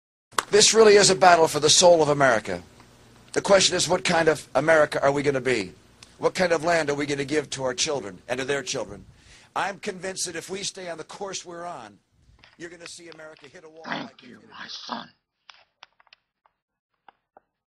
MITT ROMNEY FROM HELL
WAKE UP CHRISTIANS AND ATHEIST.  HE'S TELLING YOU SPECIFIC SCRIPTURE FROM JOSEPH SMITH.